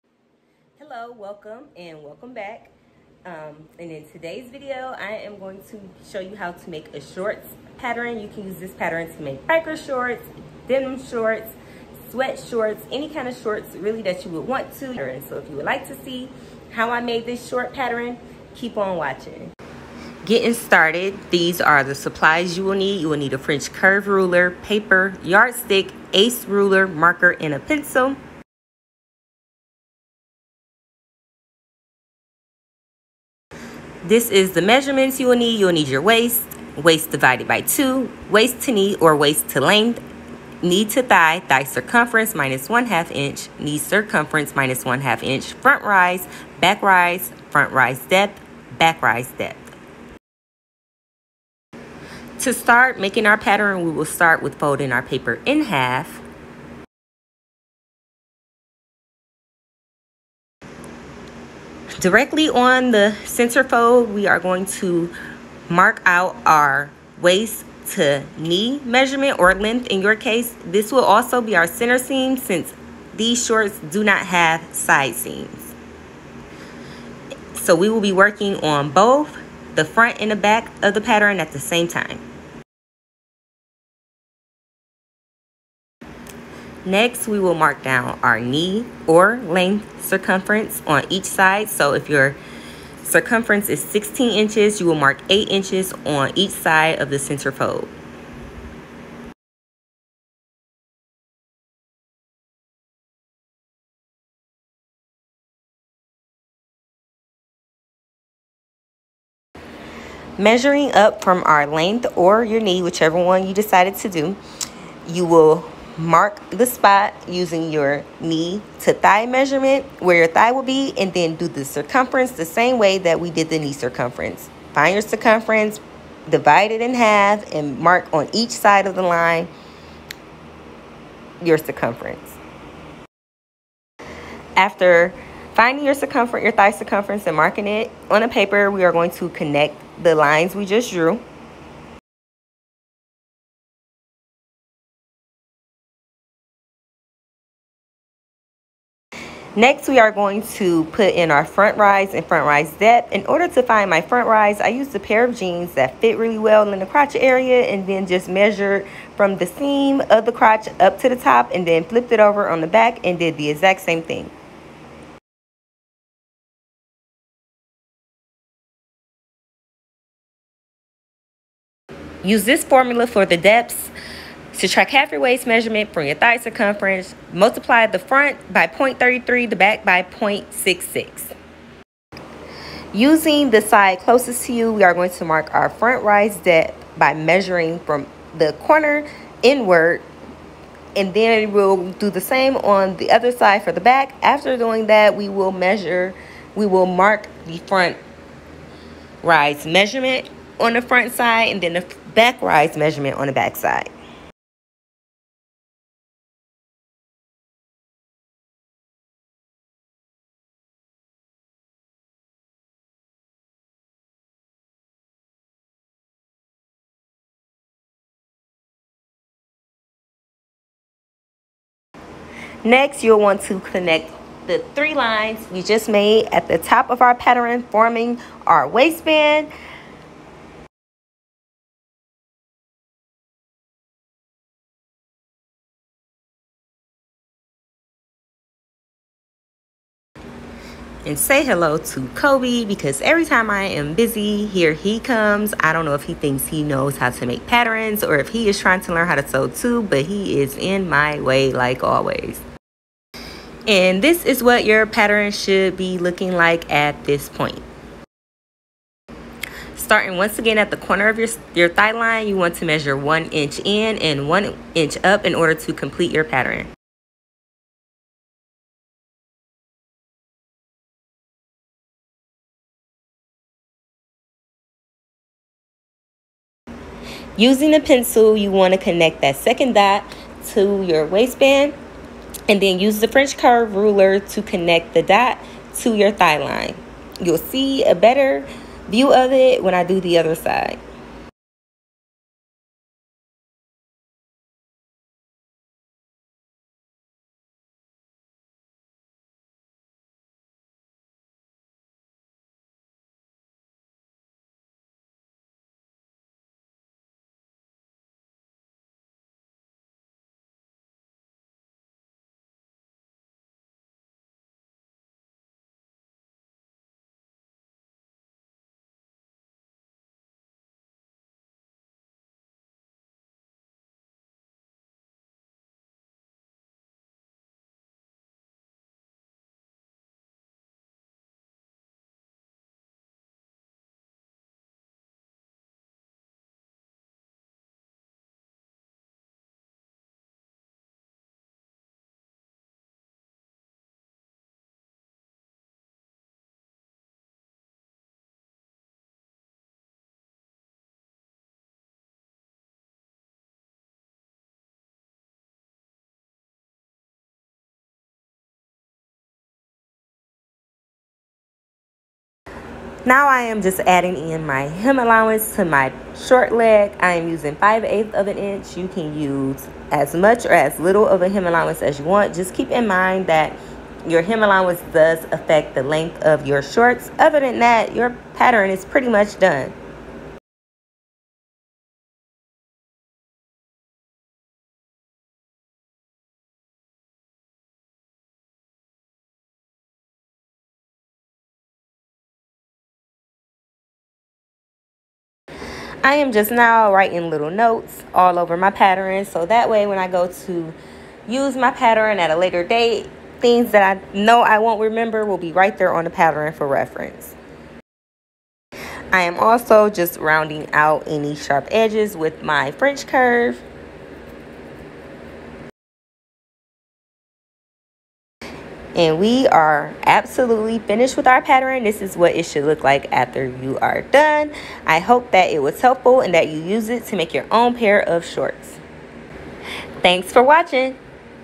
Drafting a biker shorts pattern (from scratch)
Don’t be afraid. It is super easy to do. The pattern could be used for any type of shorts depending on the fabric you use. Enjoy!